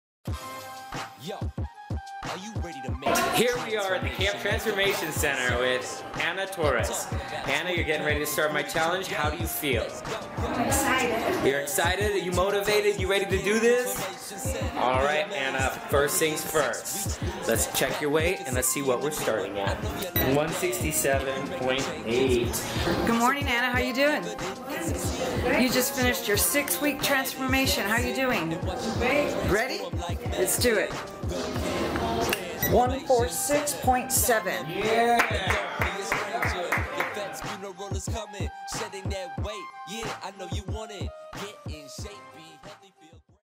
High Desert Fitness 6 Week Challenge Result - Ana Torres
We will now check the result of Ana's 6 week weight loss challenge!